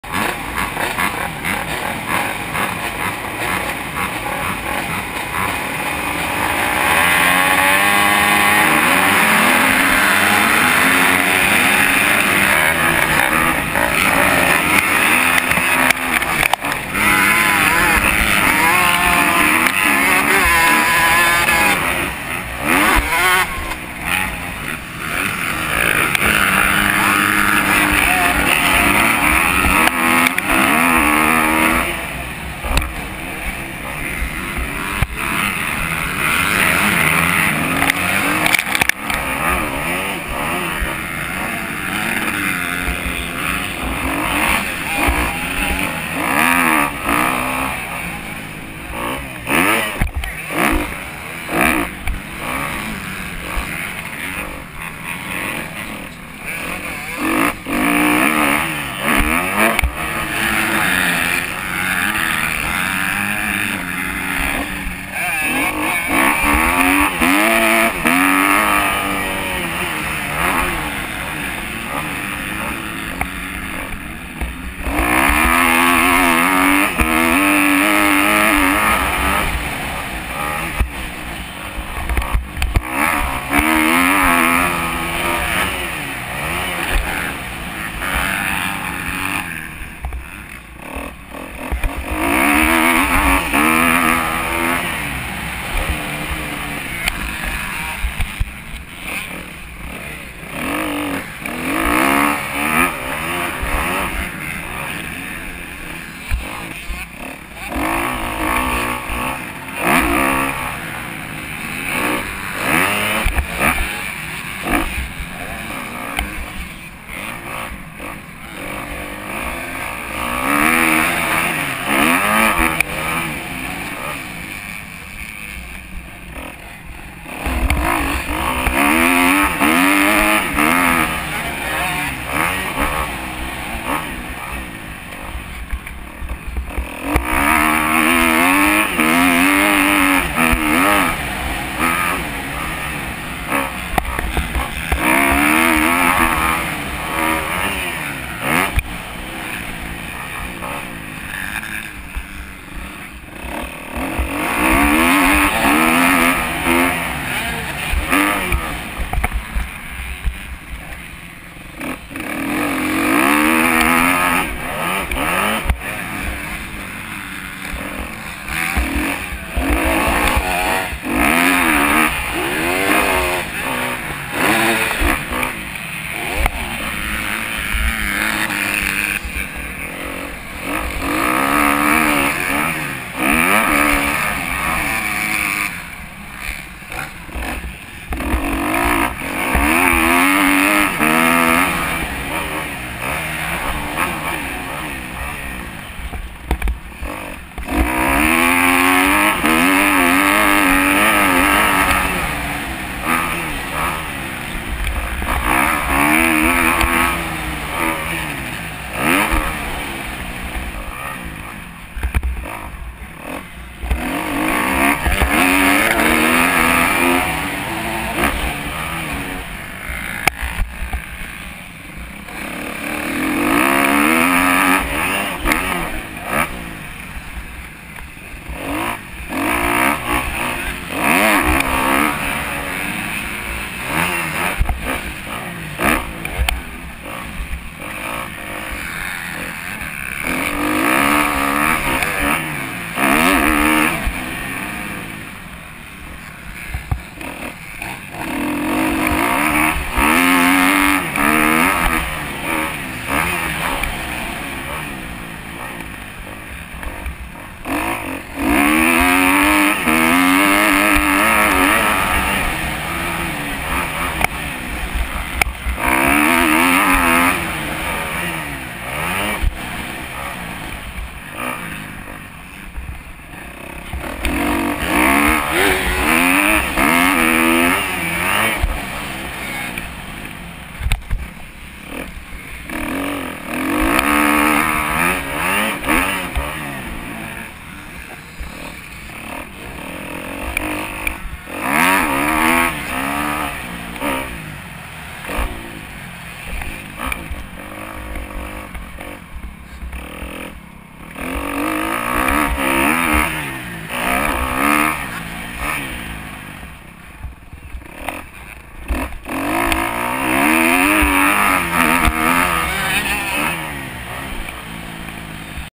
Sundance Motocross 250 Novice Moto 2
Sundance Mx Park
13 riders
250 Novice Class
7th place due to crash in the first moto.